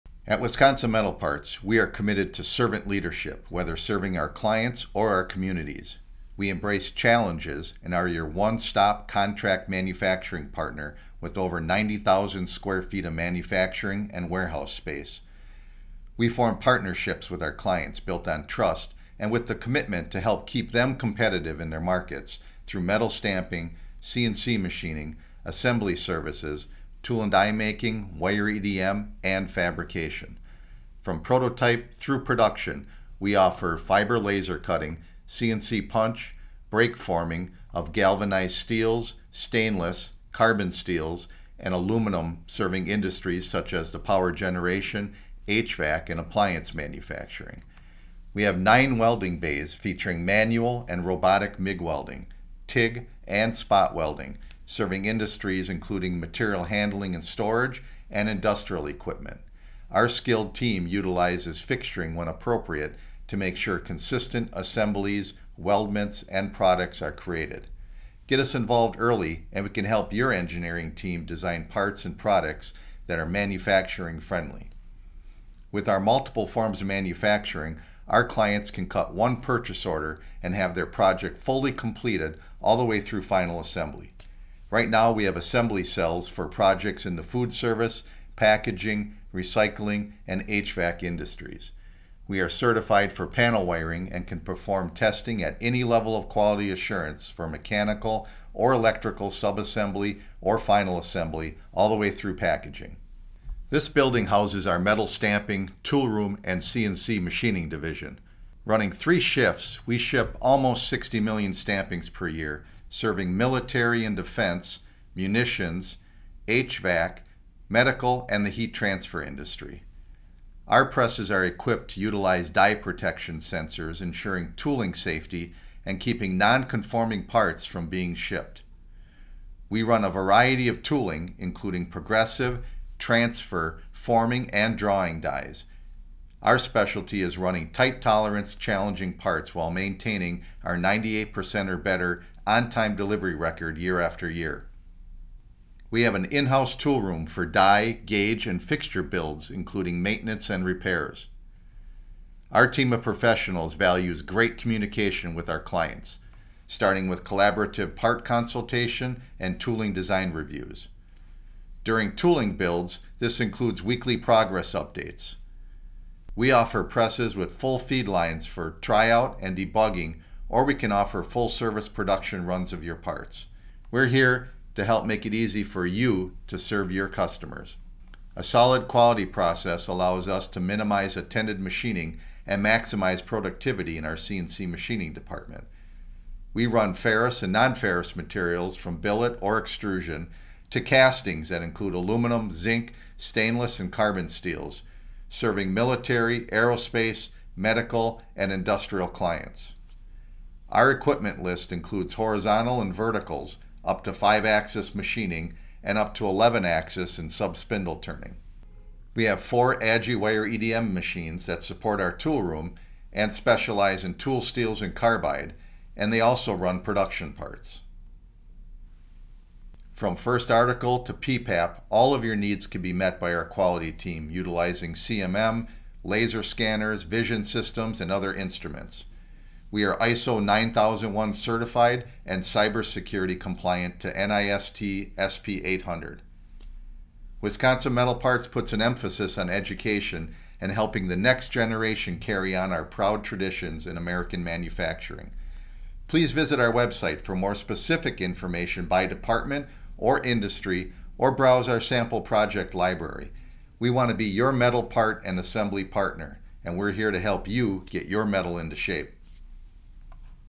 Fly-through Shop Tour - Wisconsin Metal Parts
Can't make it out for an in-person tour? Check out this video to see both our Pilot Ct and Bluemound Rd locations and learn more about how Wisconsin Metal Parts can serve you! If you want to see more, feel free to request an in-person tour on our

00:00-00:17 : Intro
00:18-01:43 : Fabrication & Assembly
01:44-03:38 : Stamping, Tooling, CNC & WEDM
03:39-03:56 : Quality & Cybersecurity
03:57-04:20 : Further Resources